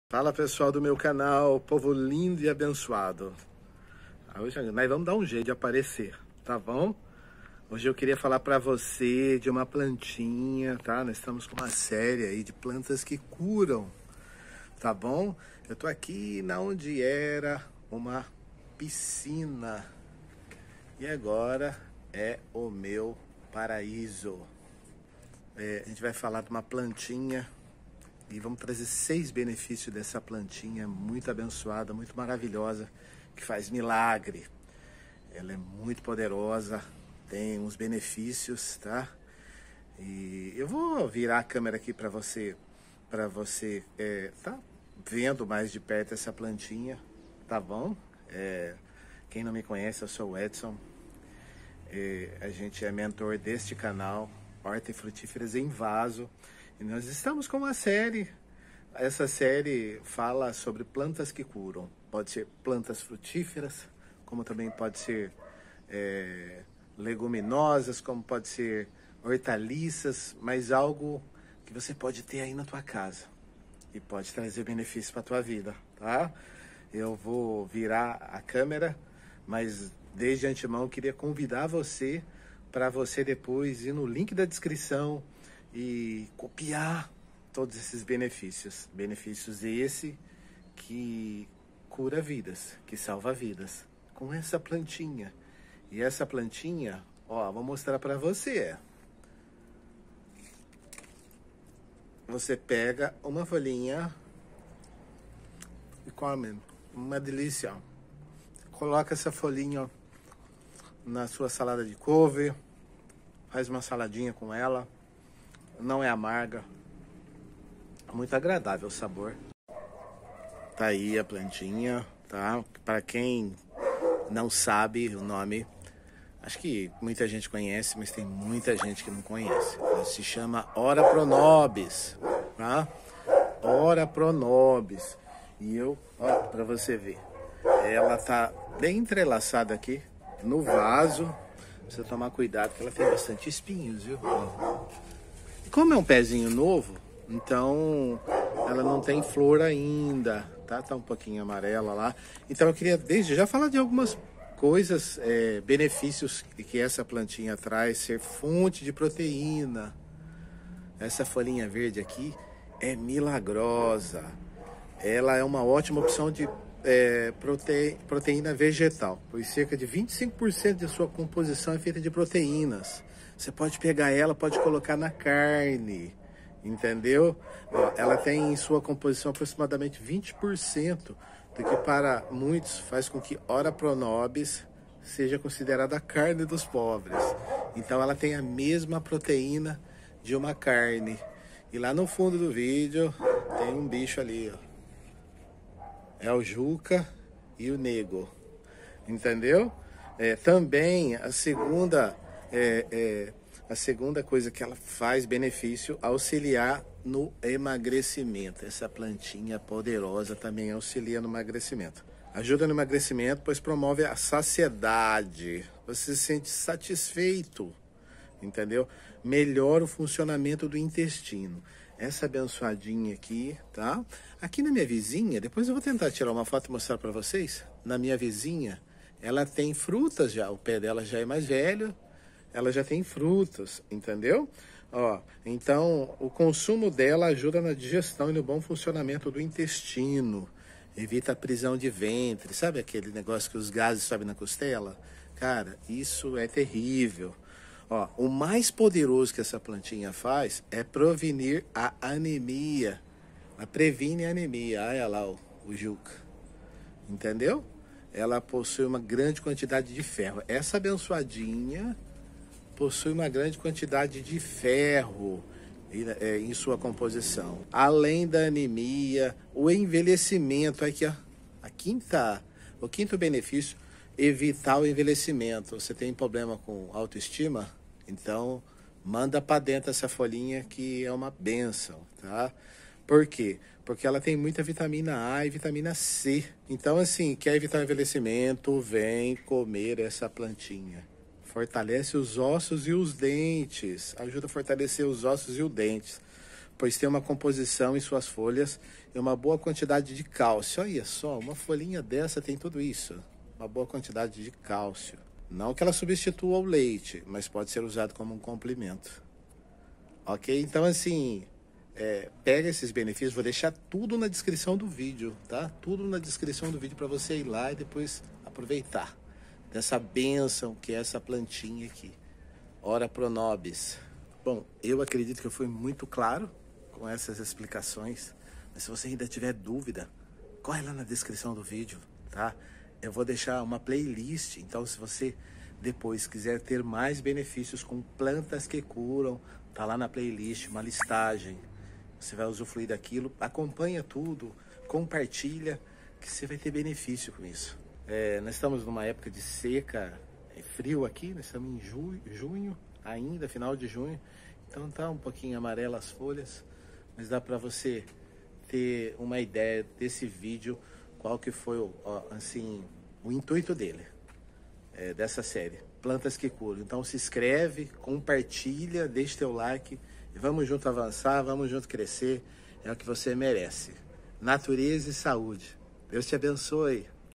Ora-Pro-Nóbis-Beneficios
Sejam todos bem vindos a esse video da série plantas que curam que com certesa vai agregar alguma coisa de bom na sua vida portanto já deixe o seu like comente e compartilhe se inscreva e seja abencoado. vou deixar aqui os 6 beneficios dessa super plantinha.

Benefícios da ora-pro-nóbis

A ora-pro-nóbis é considerada uma fonte de nutrientes barata e muito nutritiva, principalmente por ser rica em proteína, vitaminas e fibras para um bom funcionamento do intestino. Assim, alguns dos possíveis benefícios desta planta incluem:

1. Ser fonte de proteína

A ora-pro-nóbis é uma ótima opção de fonte proteica vegetal, pois cerca de 25% de sua composição total é de proteínas, a carne tem em sua composição aproximadamente 20%, o que para muitos faz com que a ora-pro-nóbis seja considerada a “carne do pobres”. Também apresenta um nível proteico alto quando comparada com outros vegetais, como por exemplo o milho e o feijão. Contém aminoácidos essenciais ao organismo, sendo o mais abundante o triptofano com 20,5% do total de aminoácidos triptofano, seguido pela lisina.
Isso faz com que a ora-pro-nóbis seja uma boa opção na dieta, para enriquecer teor proteico, principalmente às pessoas adeptas a um estilo de vida diferente, como o veganismo e vegetarianismo por exemplo.

2. Auxiliar no emagrecimento

Devido a sua quantidade proteica e por ser rica em fibras, a ora-pro-nóbis ajuda no emagrecimento pois promove a saciedade, além de ser um alimento com poucas calorias.

3. Melhorar o funcionamento do intestino

Devido a grande quantidade de fibras, o consumo de ora-pro-nóbis ajuda na digestão e no bom funcionamento do intestino, evitando a prisão de ventre, a formação de pólipos e, até mesmo tumores intestinais.

4. Prevenir a anemia

A ora-pro-nóbis possui uma grande quantidade de ferro em sua composição, sendo uma maior fonte deste mineral quando comparada a outros alimentos considerados fontes de ferro, como por exemplo, a beterraba, a couve ou o espinafre. No entanto, para a prevenção da anemia, o ferro deve ser absorvido junto com a vitamina C, outro componente presente em grande quantidade neste vegetal. Sendo assim, as folhas de ora-pro-nóbis podem ser consideradas um bom aliado para prevenir a anemia. Confira outros alimentos que também ajudam a prevenir a anemia.

5. Evitar o envelhecimento

Devido a grande quantidade de vitaminas com poder antioxidante, como por exemplo as vitaminas A e C, o consumo de ora-pro-nóbis ajuda a diminuir ou até mesmo inibir os danos causados nas células. Isto ajuda a prevenir o envelhecimento precoce da pele, auxilia na saúde do cabelo e unhas, além de melhorar a visão. Por ser rica em vitamina C, também auxilia no fortalecimento da imunidade. Conheça outros alimentos ricos em vitamina C.

6. Fortalecer os ossos e dentes

A ora-pro-nóbis ajuda a fortalecer os ossos e dentes, pois tem na composição de suas folhas uma boa quantidade de cálcio, são 79 mg em cada 100 g de folha, o que é um pouco mais da metade do leite, que oferece 125 mg por 100 ml. Embora não substitua o leite, pode ser usado como um complemento.